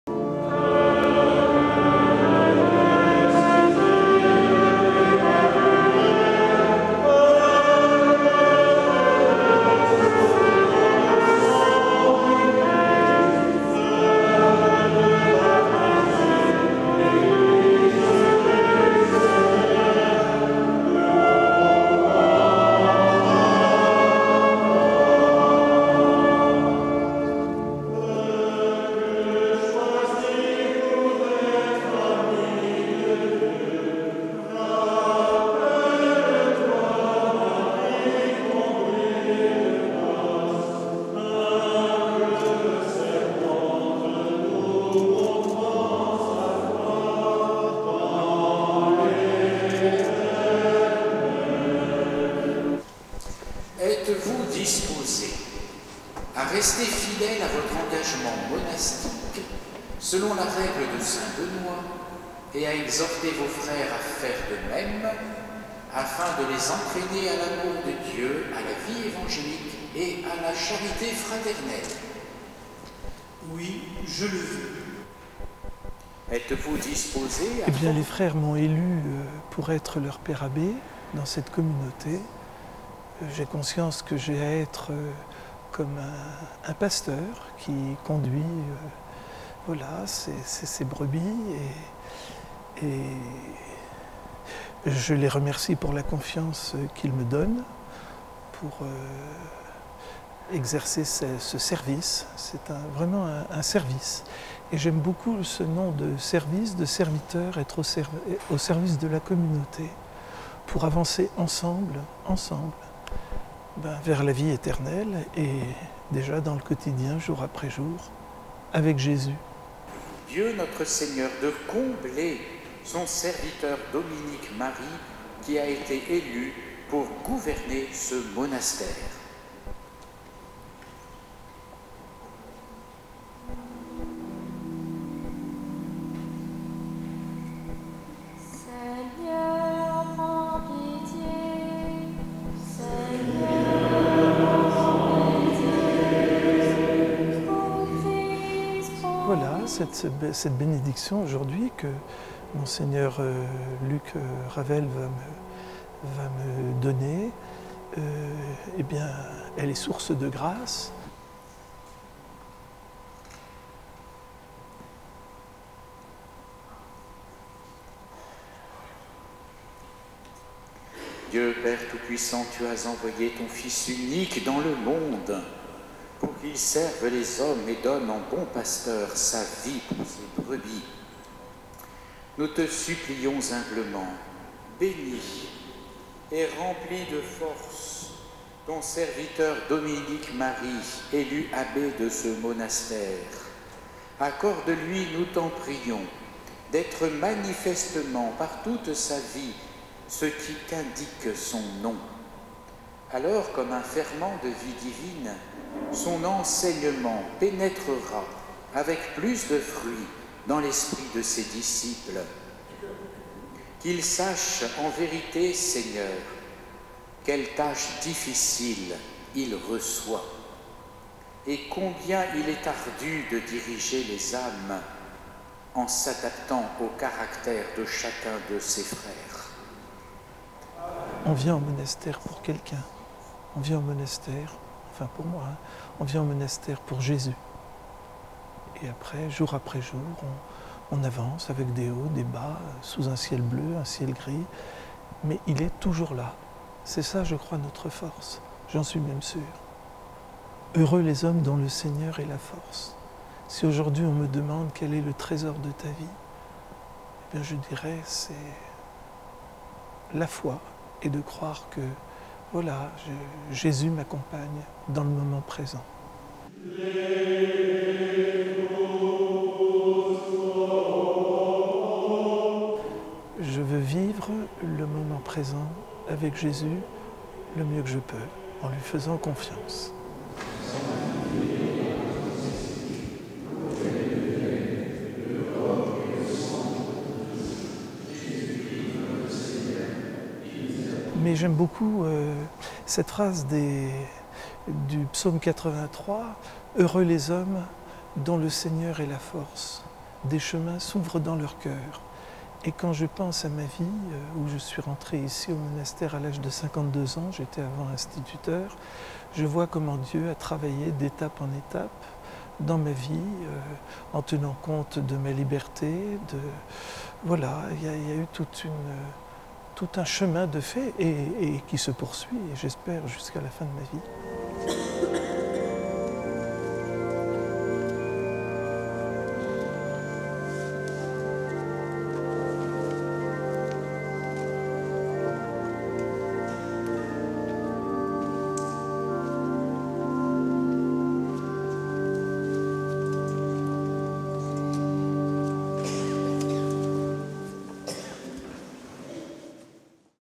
Bénédiction abbatiale de frere Dom Dominique Marie
BENEDICTION ABBATIALE DE                      Dom Dominique-Marie SCHOCH
 Dom Dominique-Marie SCHOCH prend en charge l’abbaye Notre-Dame d’Oelenberg avec ses huit moines, les  14 religieuses cisterciennes de Baumgarten ainsi que la communauté de cinq autrichiens à Engelszell. Tous ces religieux obéissent à la règle de SaintBenoît           Reportage :  Cécile HANS – Jean Pierre BERTRAND
Durée 05 minutes et 51 secondes